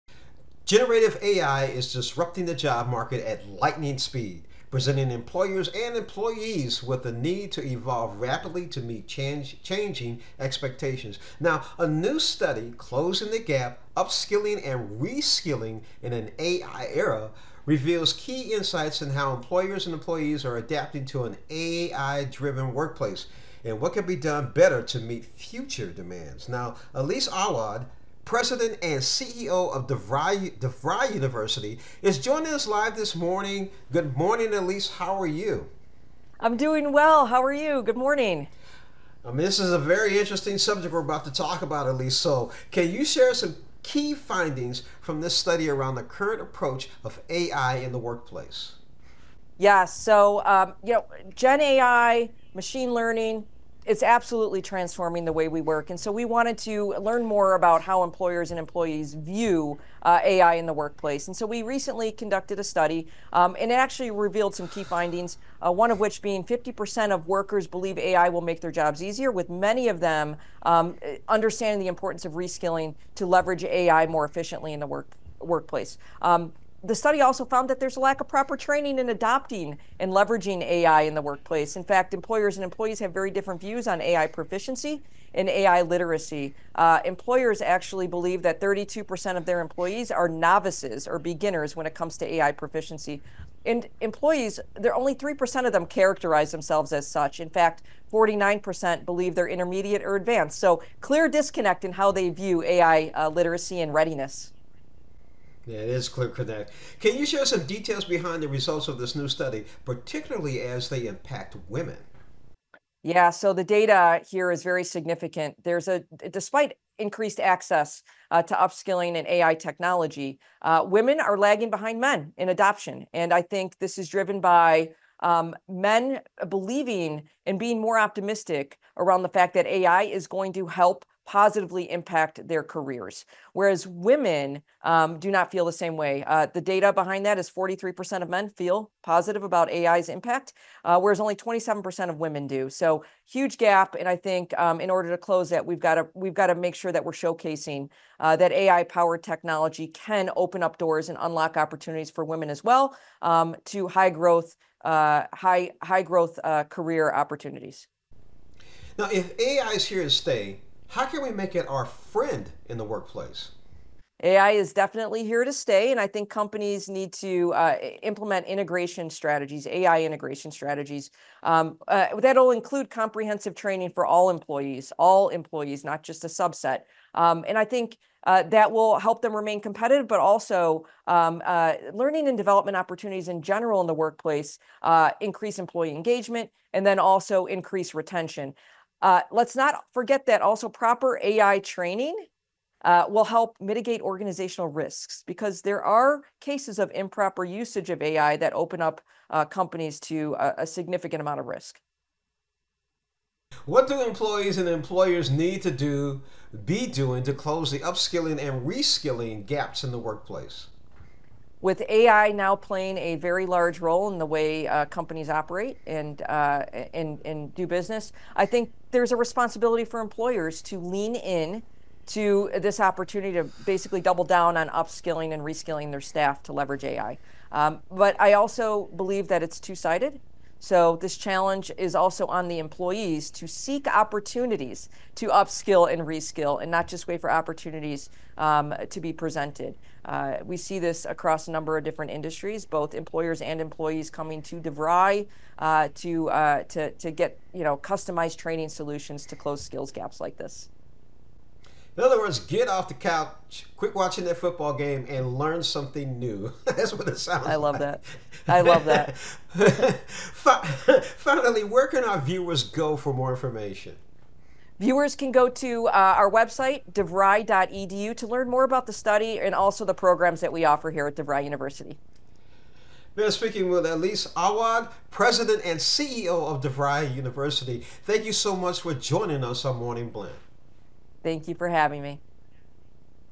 Closing the AI Gap: Can Workplaces Adapt Fast Enough to Keep Up?
Generative AI is disrupting the job market at lightning speed, presenting both employers and employees with the need to evolve rapidly to meet changing expectations. But a new study reveals some significant gaps in perception that may be hindering growth in the workplace.

Surveying data from more than 1,500 employees and 580 hiring decision-makers, the report dives into the skills development opportunities available to employees and seeks to understand how employers AND employees are addressing the AI elephant in the room.

Some key insights:

Workers are still saying one thing and doing another. 30% of employees report that they face no barriers to upskilling, nearly 9 in 10 employers (87%) say they offer upskilling opportunities, yet only 55% of employees are taking advantage of them.
Employees think their AI skills are better than their employers do. Employers estimate about 32% of their employees are “merely novices,” while only 3% of employees would describe themselves that way.
Women are falling behind in AI skills, less likely to see AI as helpful. Only 41% of women describe their AI proficiency as “intermediate or higher,” compared with 55% of men, and only 27% of women see AI as a way for them to get ahead, while 43% of men hold to that belief.

Elise Awwad, president and CEO of DeVry University, will be available to offer her insight on this new data. How can it help workplaces trying to decide next steps to close the AI gap and stay ahead of the curve?